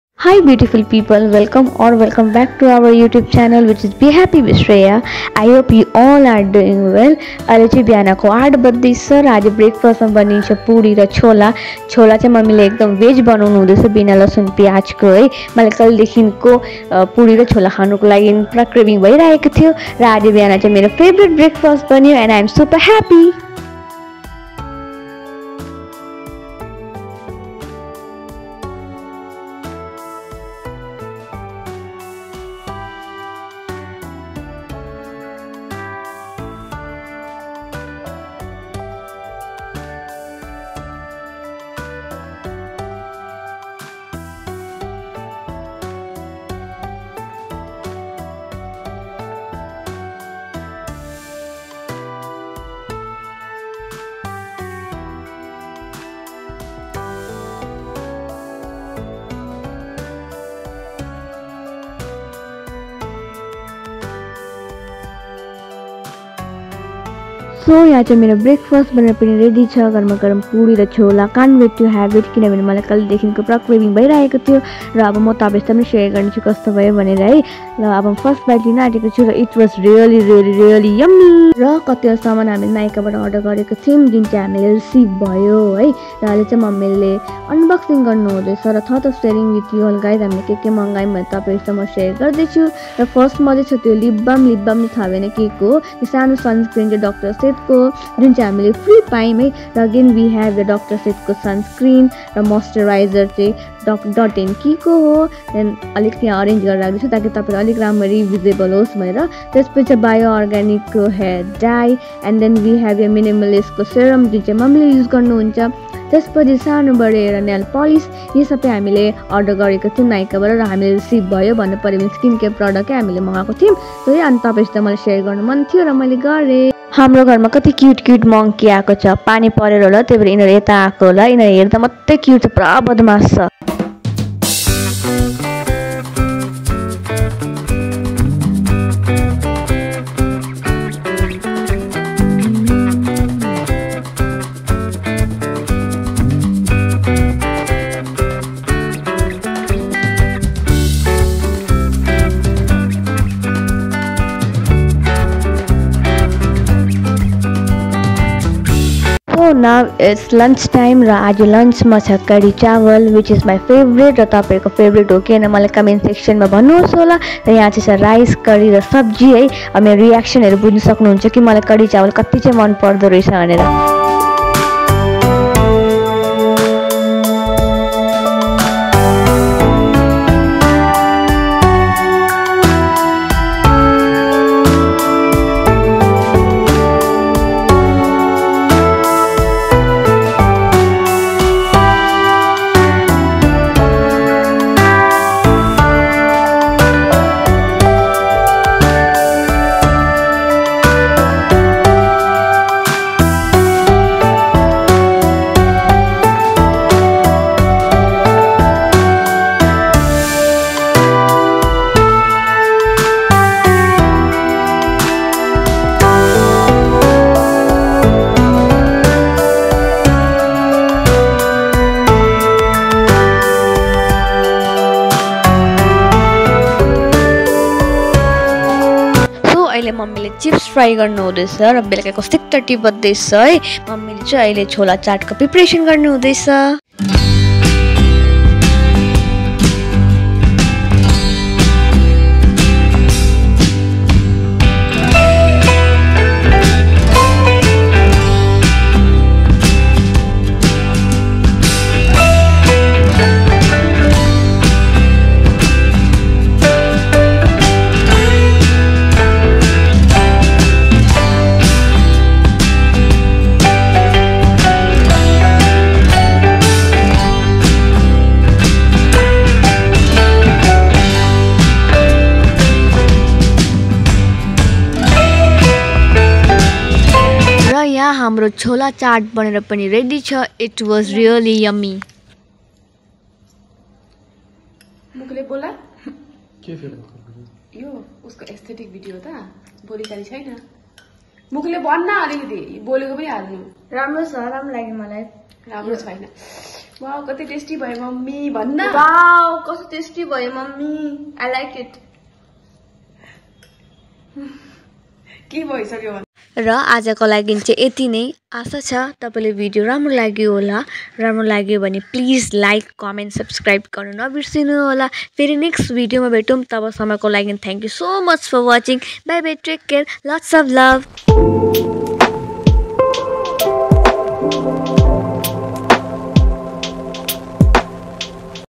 A Day In My Life😊😊|| Kadhi Chawal || best breakfast puri and chola 😋❤️ ||A special day 😊🌸
A Day In My Life😊😊|| Kadhi Chawal || best breakfast  puri and chola 😋❤️||special day